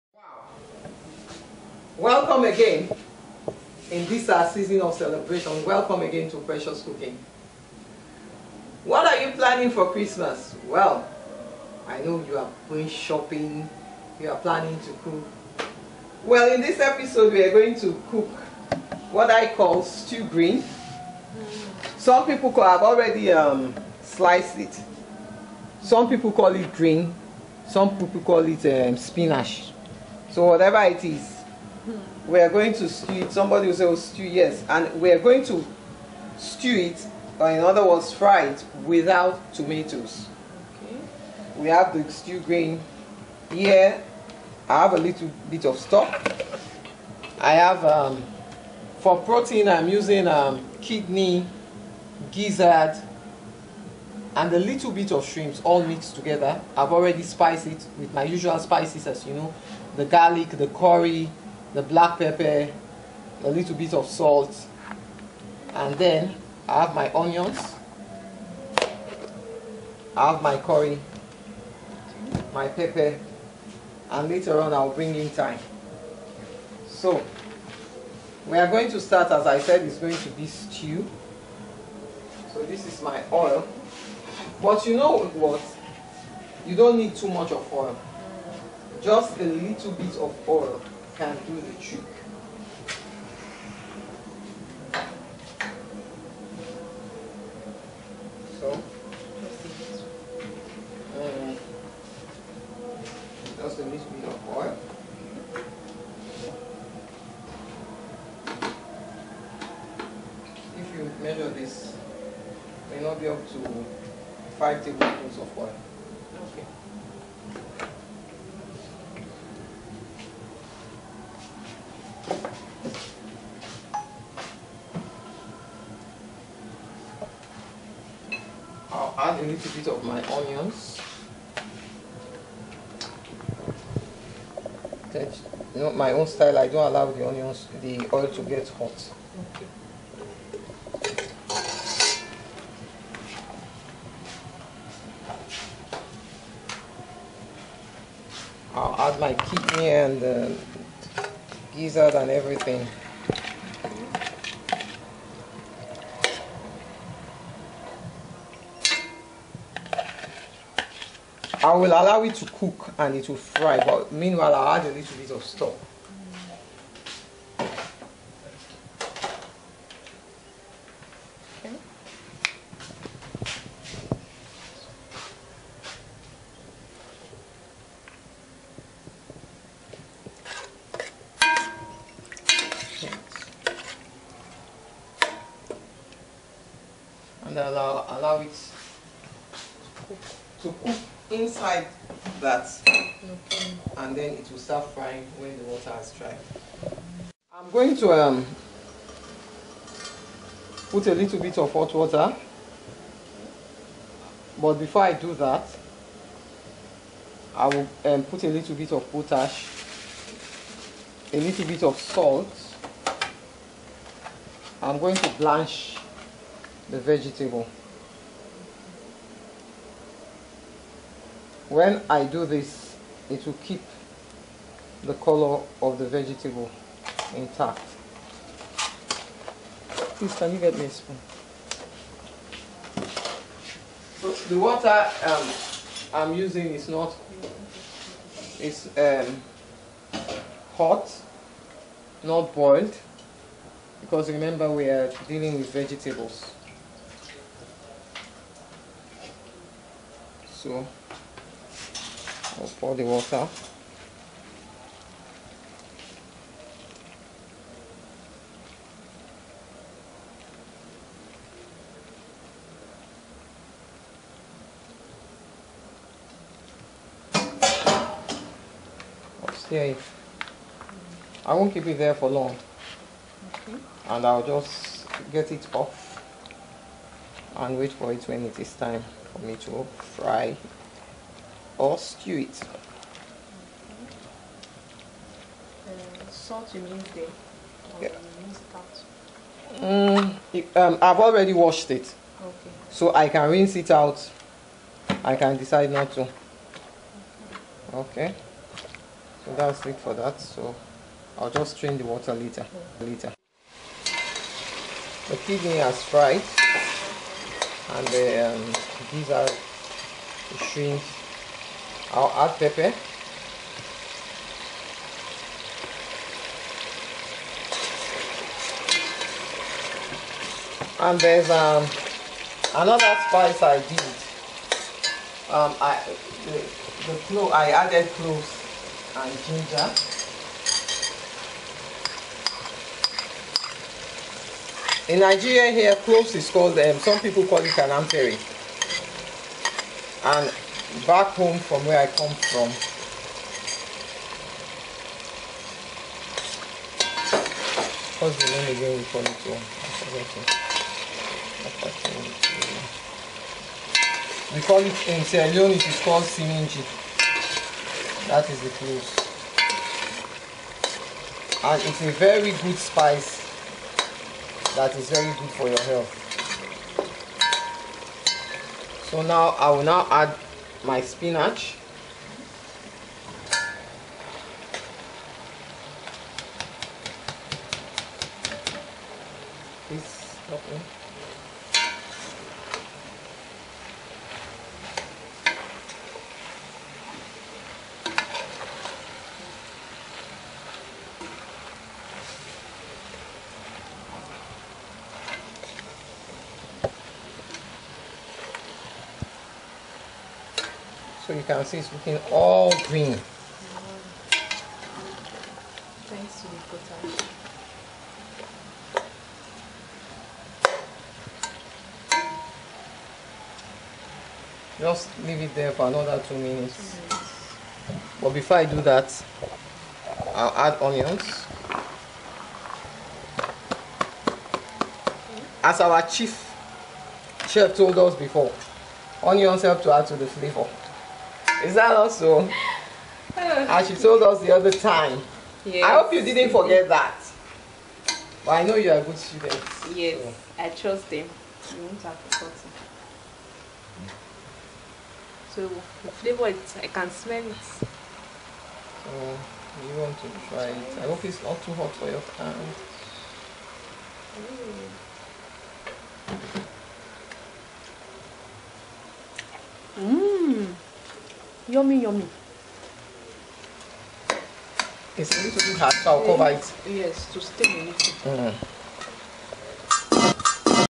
HOW TO MAKE STEW GREEN/SPINACH
Ingredients for making STEW GREEN:
Green Vegetable
Kidney
Gizzard
Pepper
Black Pepper
Garlic
Onions
Curry
Salt
Oil